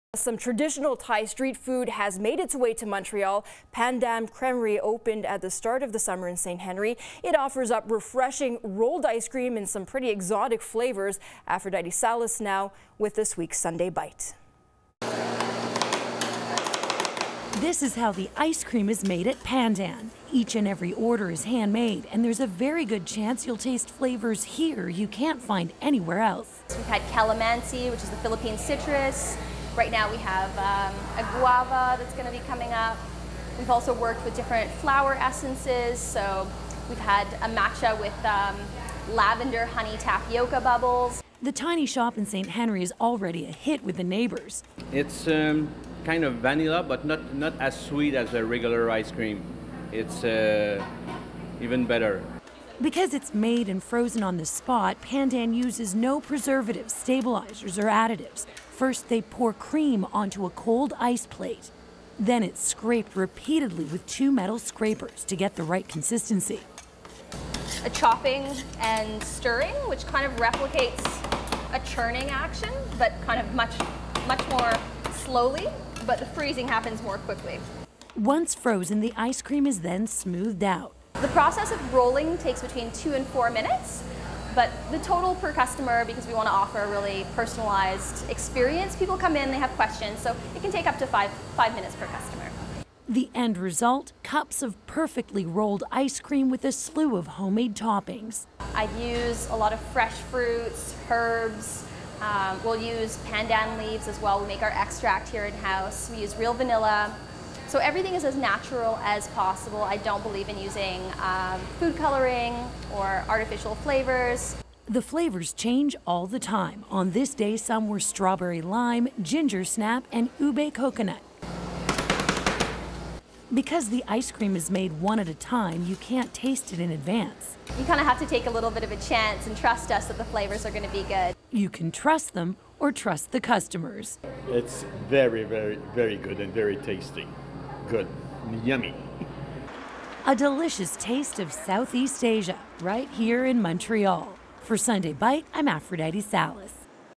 Sunday bite: Thai-style ice cream with exotic flavours
Pandan Cremerie offers up refreshing rolled ice cream in some pretty exotic flavours that make it a Thai treat.